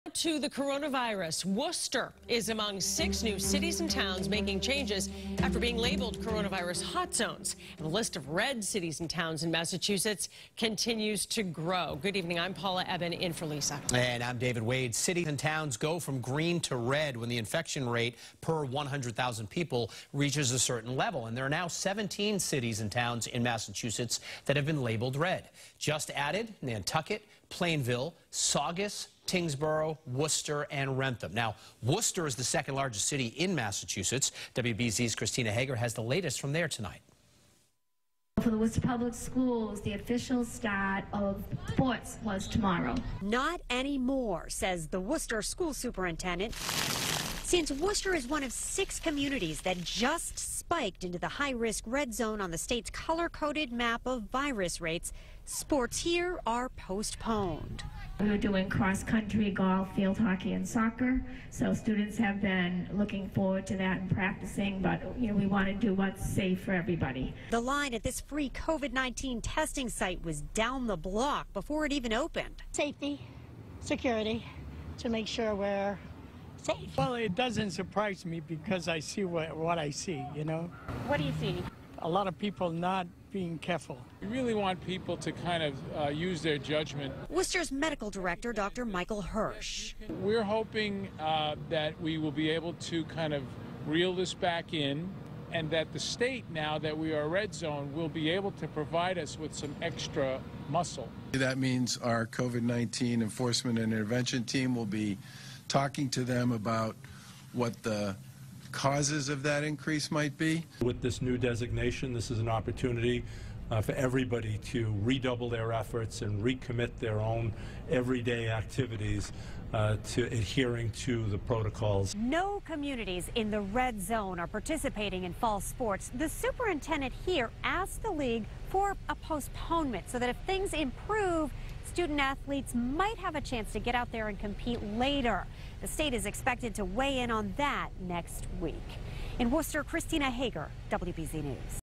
Worcester Among 6 New Communities Listed As High Risk For Coronavirus
School sports have been postponed and enforcement is being stepped up in Worcester. WBZ-TV's Christina Hager reports.